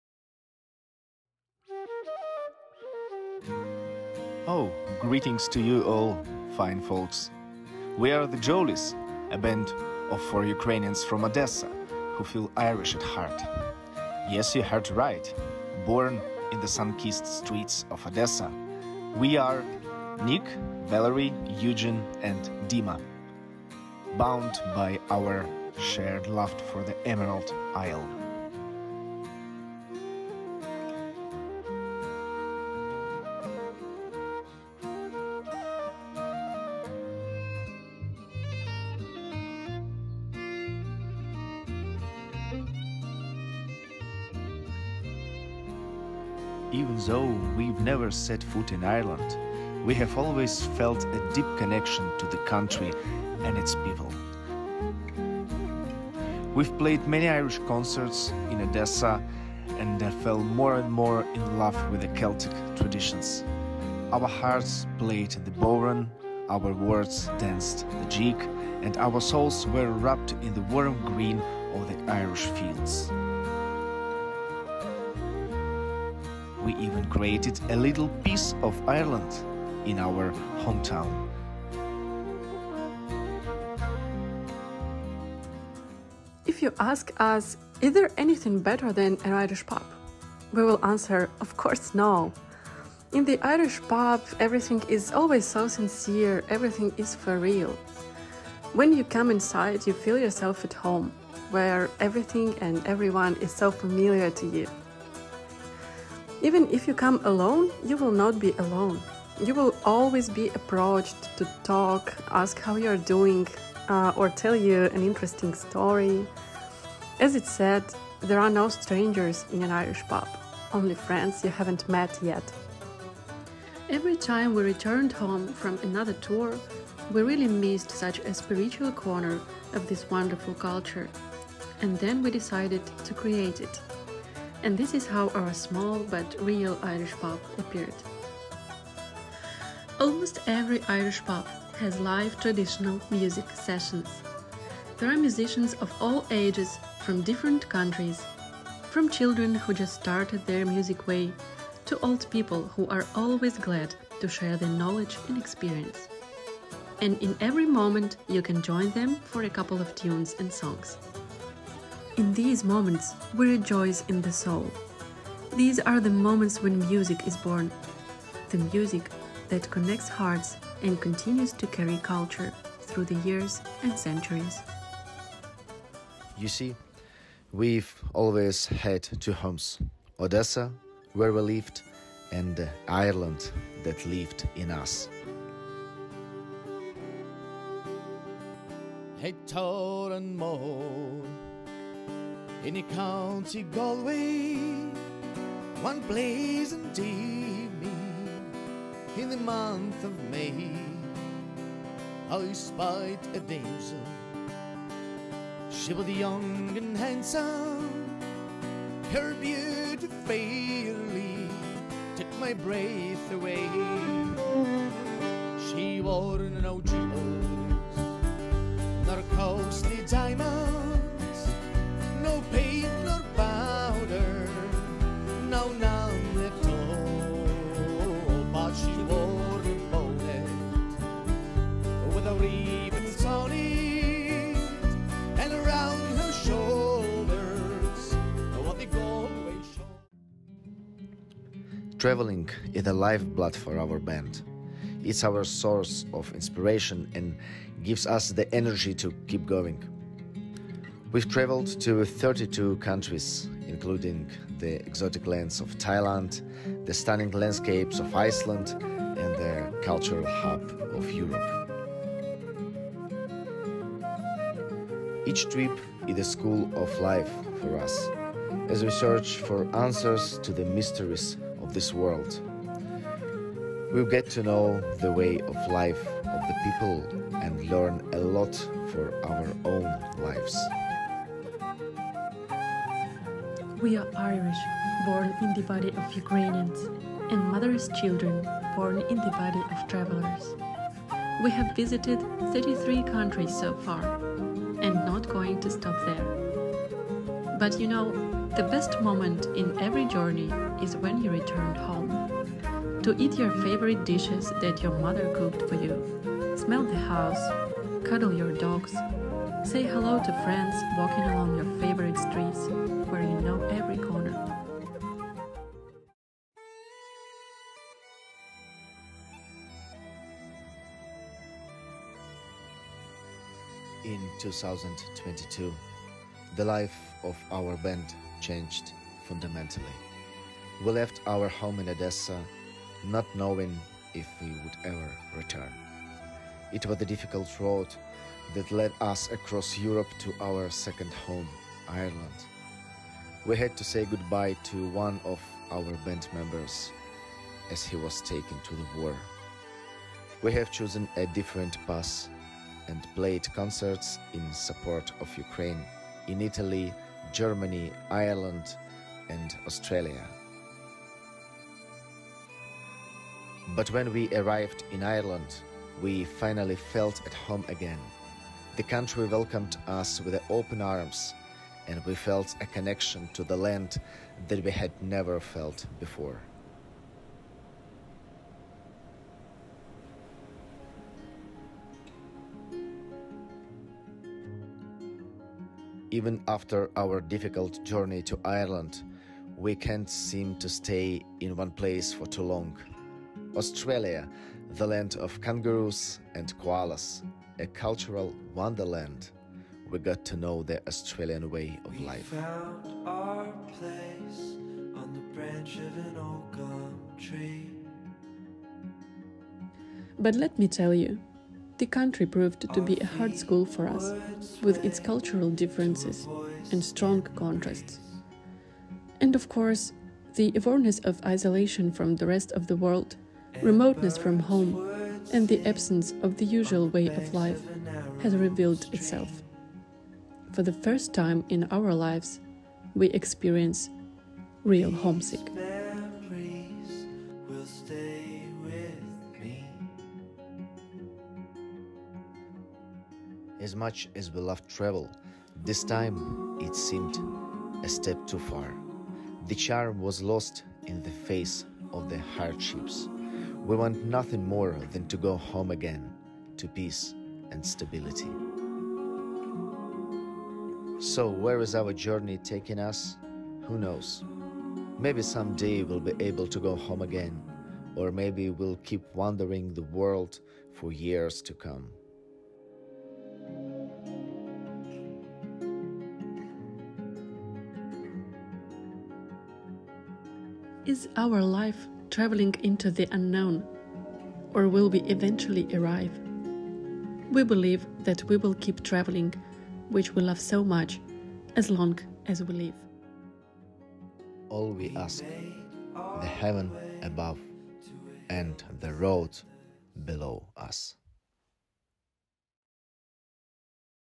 The Jolly's - The year of wandering
A little story about The Jolly's - a bunch of 4 Ukrainians who feel Irish at heart.
A bit of our background, tours, travels, life in Ireland and the year of the war, that made us wandering around the world...